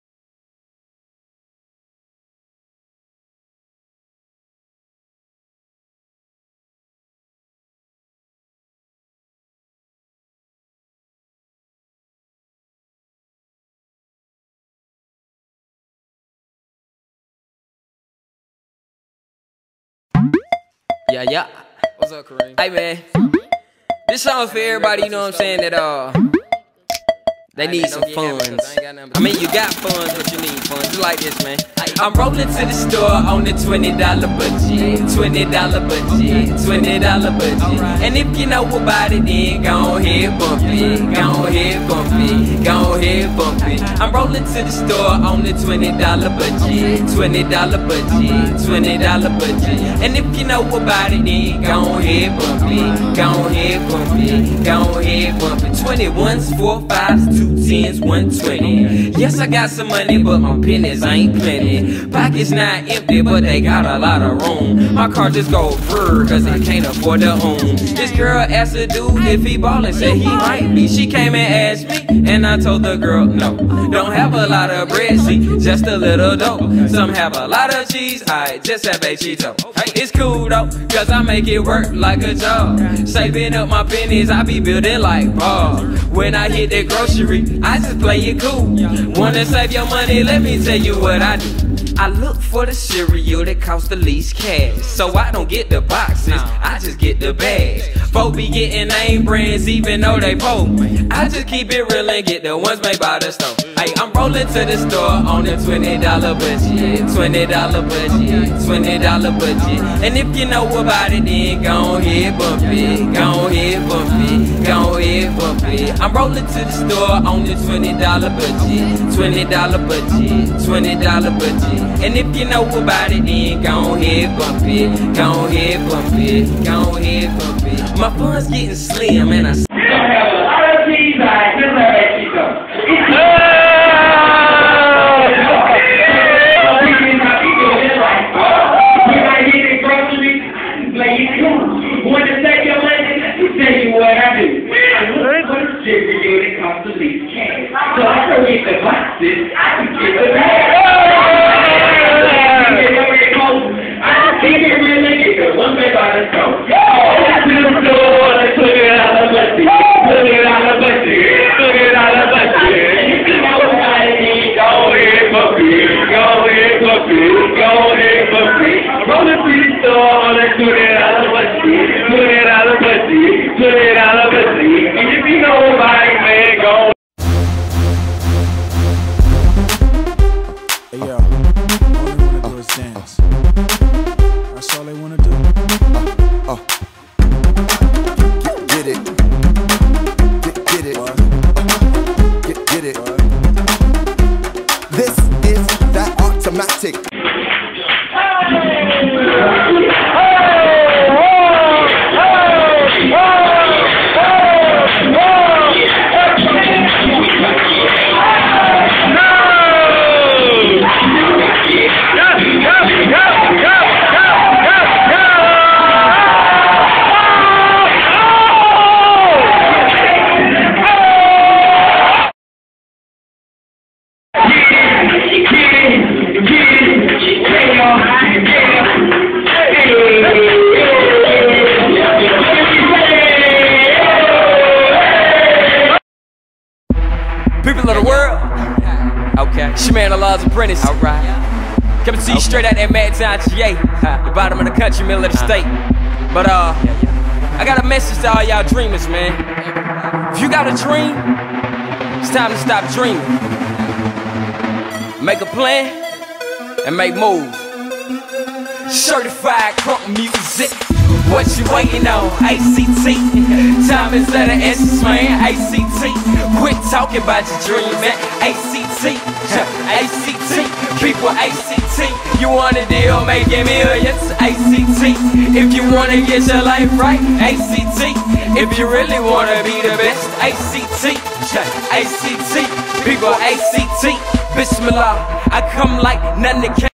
ACT @the AUC
The first episode from the black Curve files follows the events of the Night of Feb. 7th at Jazzman's Cafe on Morehouses campus. All that follows is the ish.